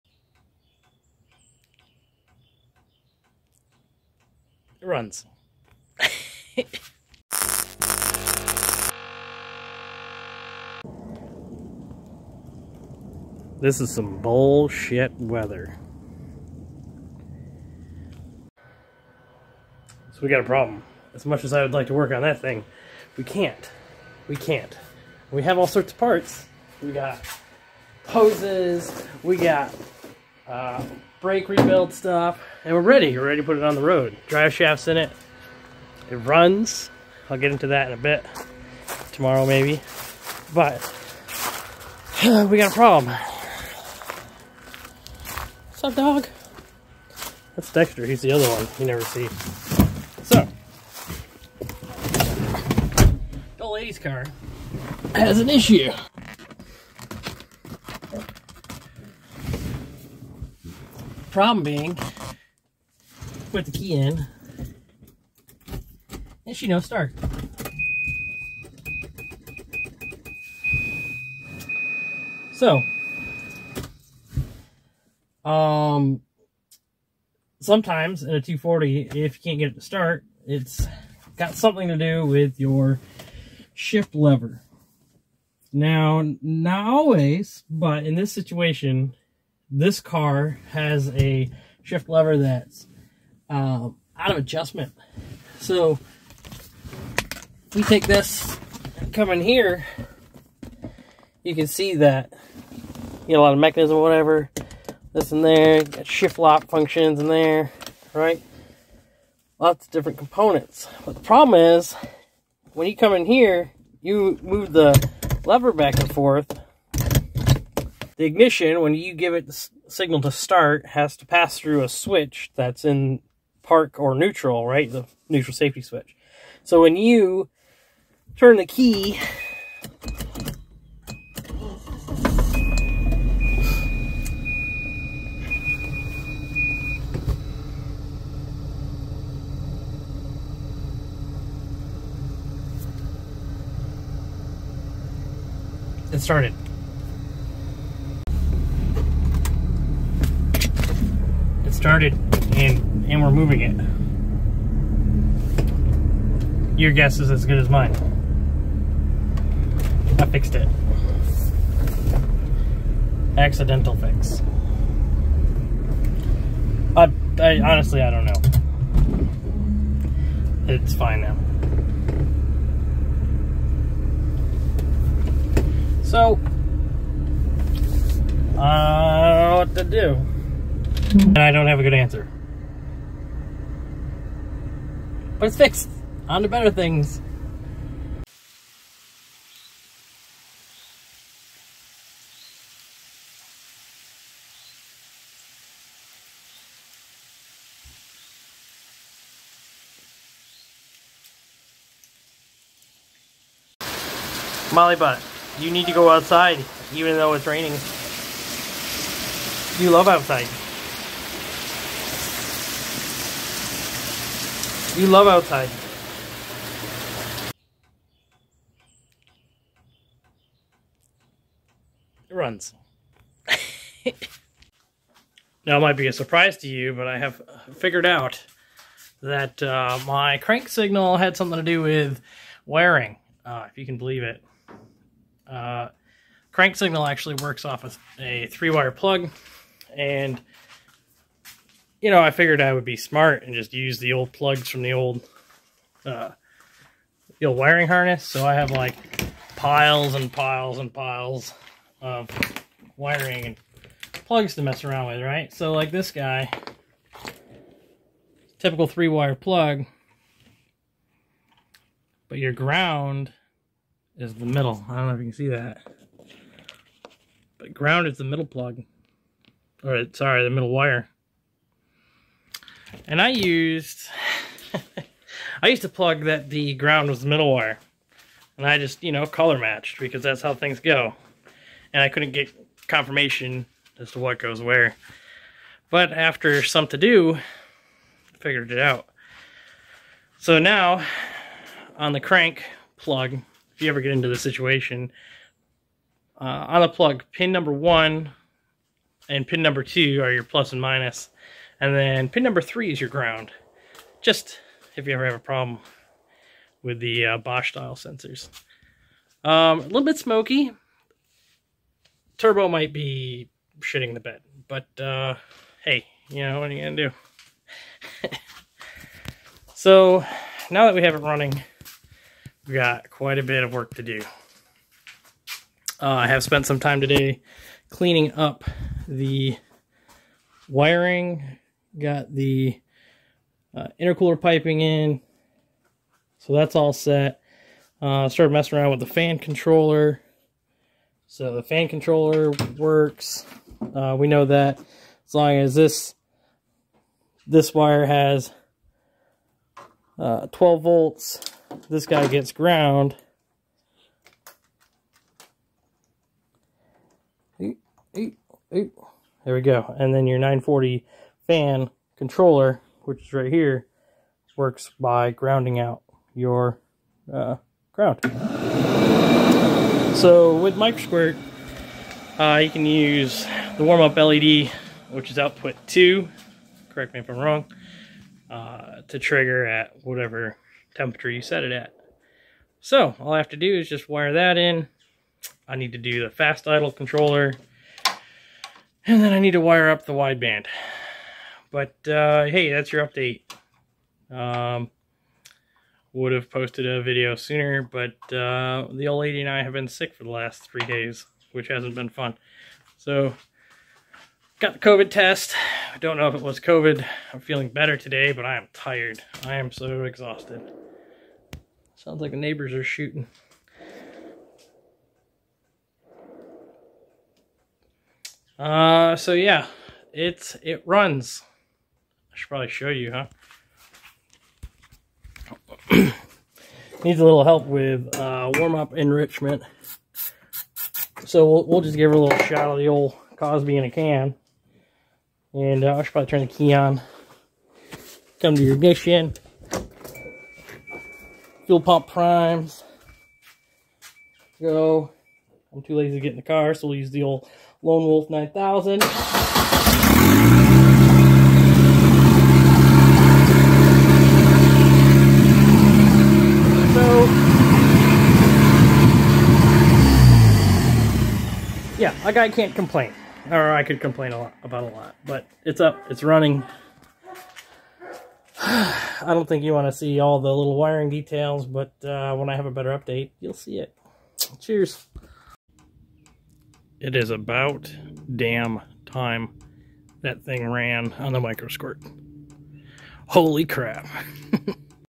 Volvo 245 TURBO Swap! Running on a Microsquirt Stand Alone ECU!
The lady's 245 has a finicky issue with starting. Guess where the problem is!?

Turbo 245 is finally running on the Microsquirt ECU!